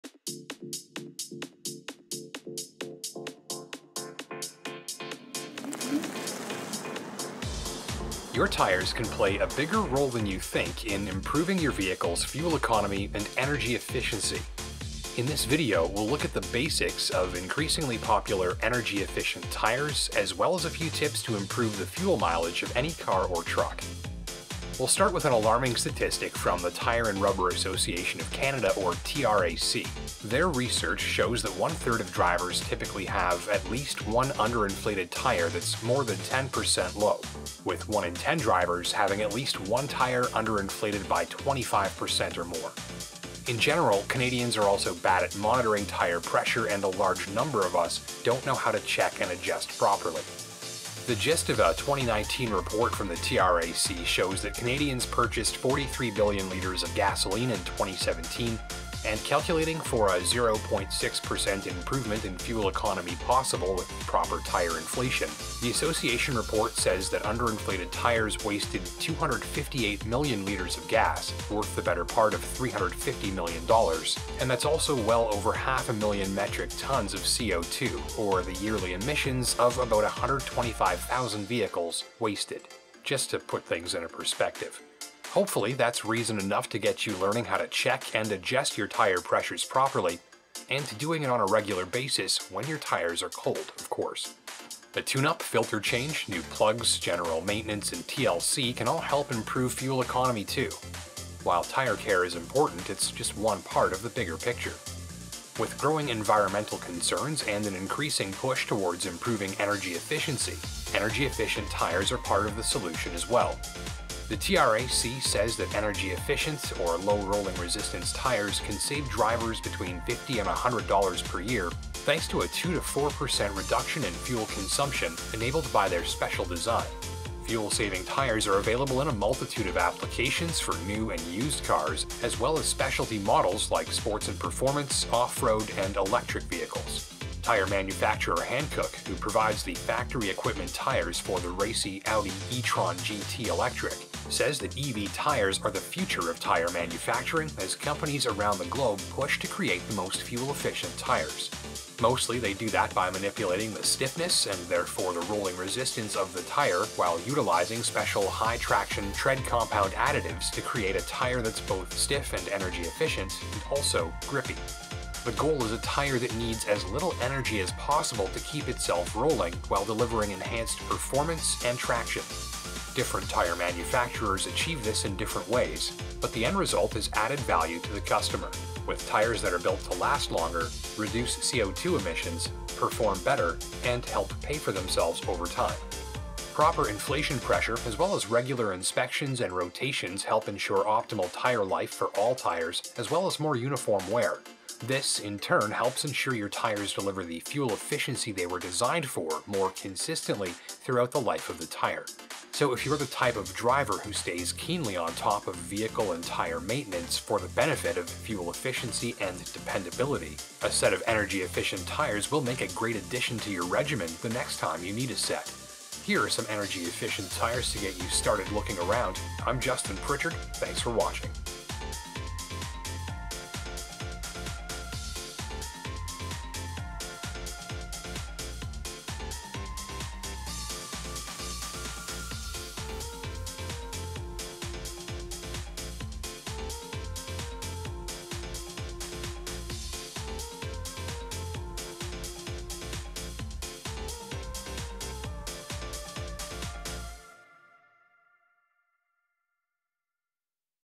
Are Fuel-Saving Tires Worth It? (& How to Save Fuel With Any Tire!) | Driving.ca
Today’s shopper is more eco-savvy than ever, and products that offer efficiency and energy-savings are attracting plenty of attention.

As automakers shift to electrification and design vehicles around maximum energy efficiency, tires play heavily into the equation. Energy-efficient tires have been around for a few years, and today's tire manufacturers are offering more innovative solutions across more types of tires than ever.

In 2021, the design, compounds, and technologies that marry the two continue to evolve, giving shoppers more choices and value. Energy-efficient tires are increasingly common on new cars, and plenty of options are available for shoppers driving older vehicles, too.

Follow Driving!